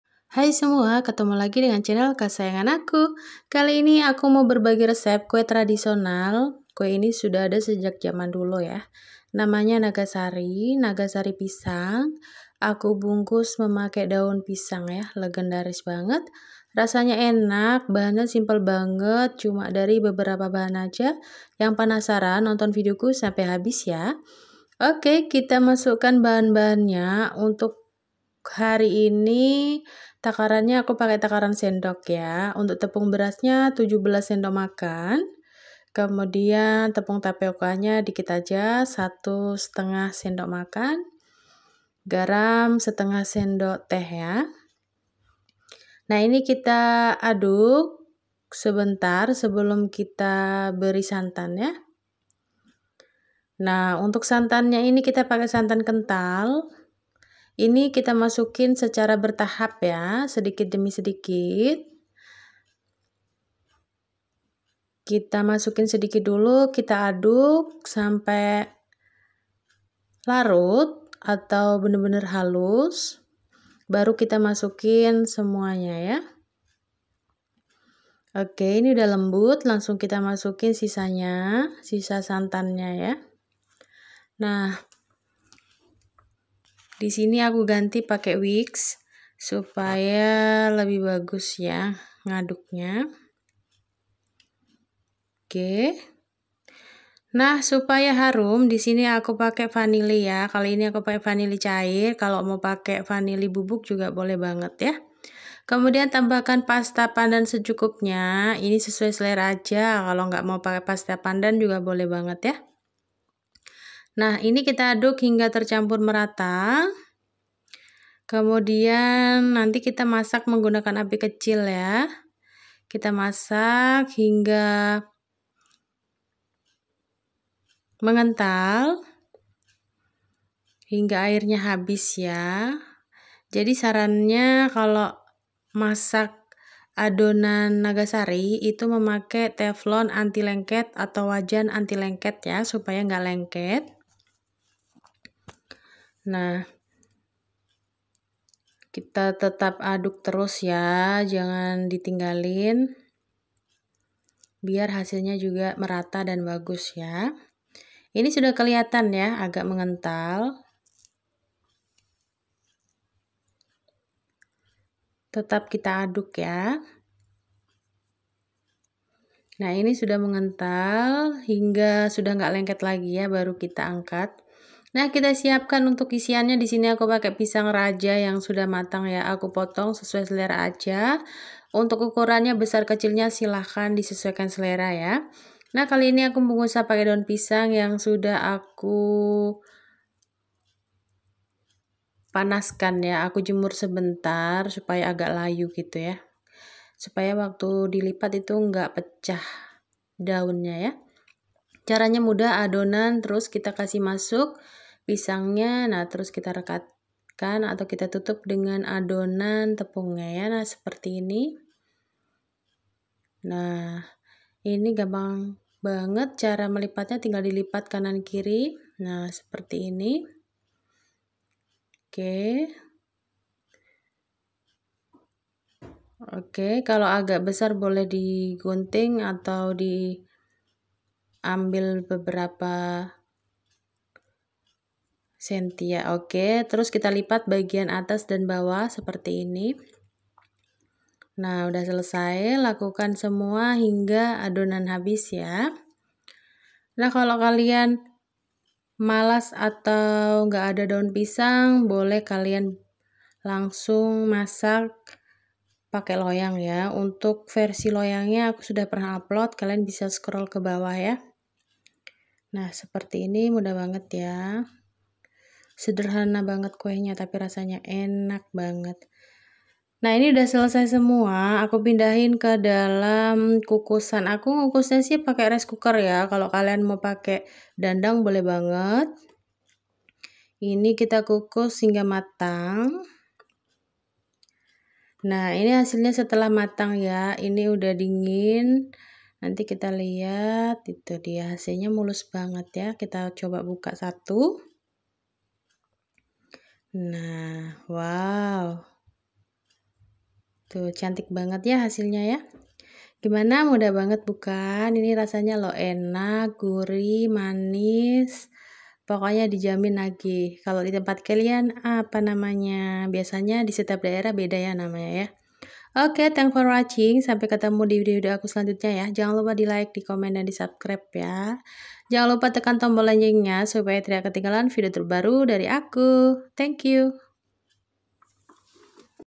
RESEP KUE NAGASARI PALING ENAK DAN MUDAH || ENAK BANGET
hai semua ketemu lagi sama aku
kali ini aku mau bikin kue tradisional
kue nagasari Pisang
enak banget
caranya juga mudah banget

bahan bahan lengkap ada di vidio yah🥰